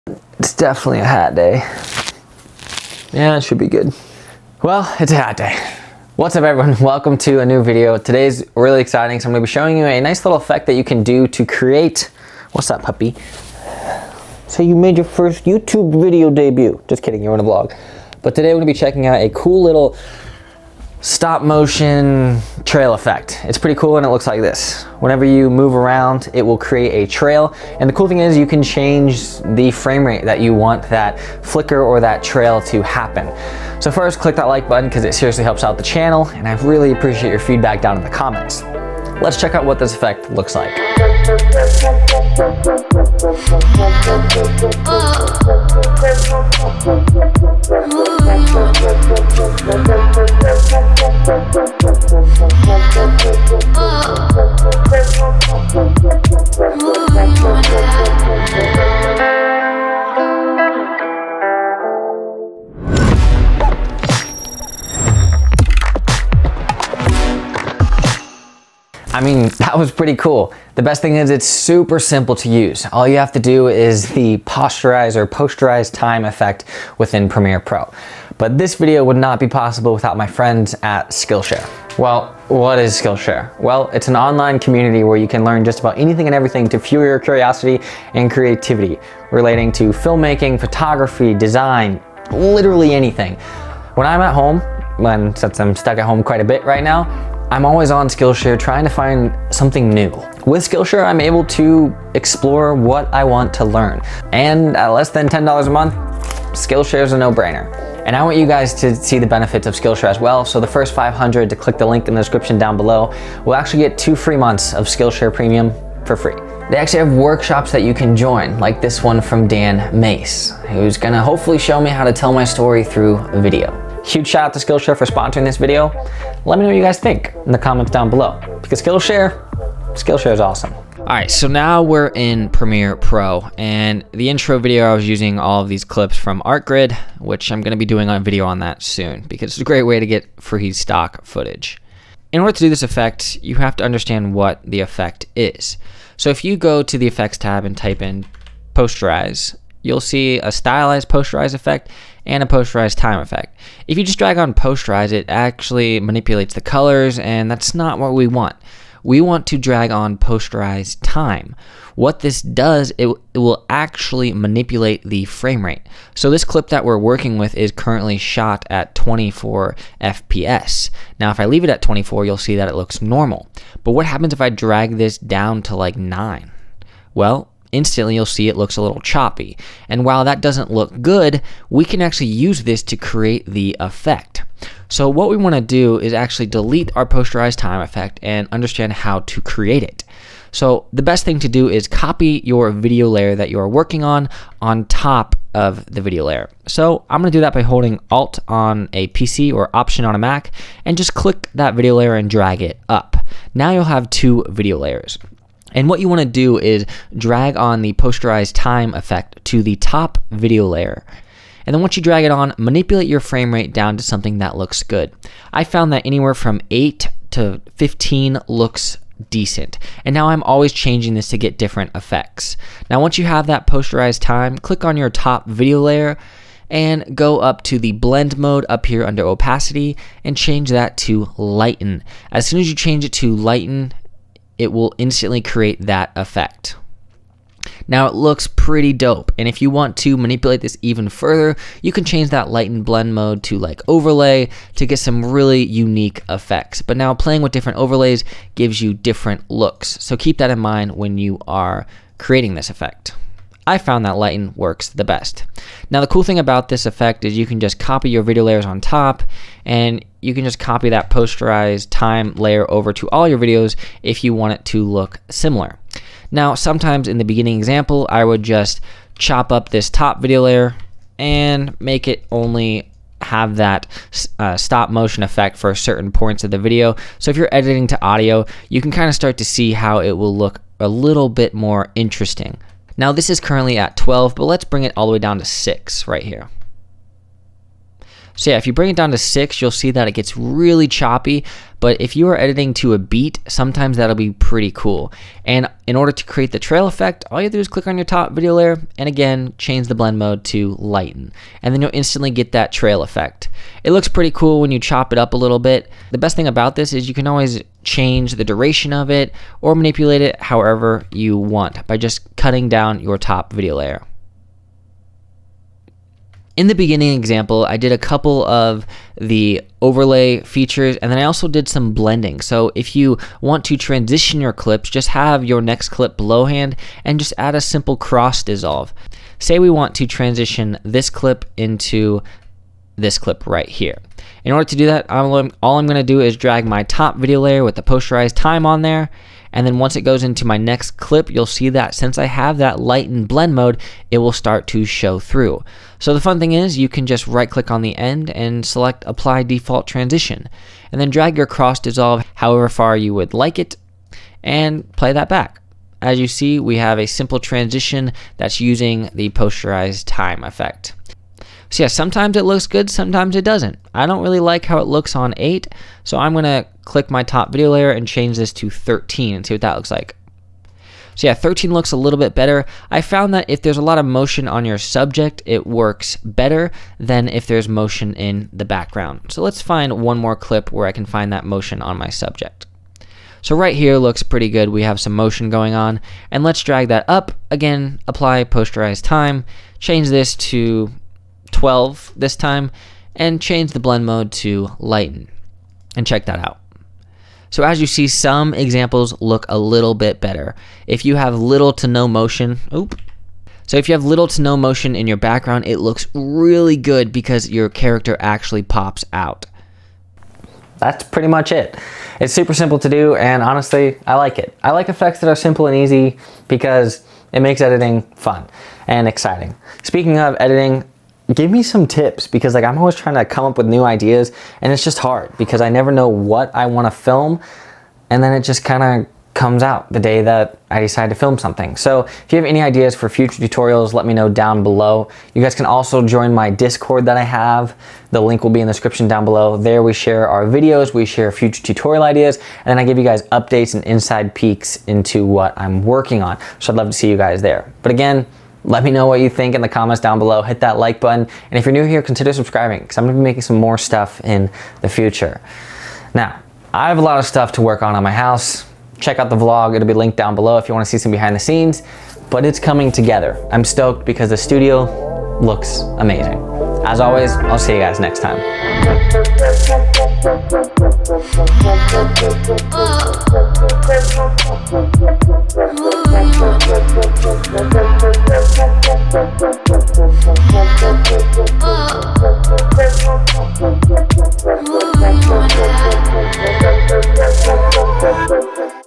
STOP MOTION Trail Effect | Premiere Pro
Have you ever wondered how to create a stop motion trail effect in Premiere Pro? Well, this video will go over some tips and tricks to show you how to create a unique effect in a couple steps! It'll even show you how to create stop motion on any framerate you would like!

|   -CONNECT-   |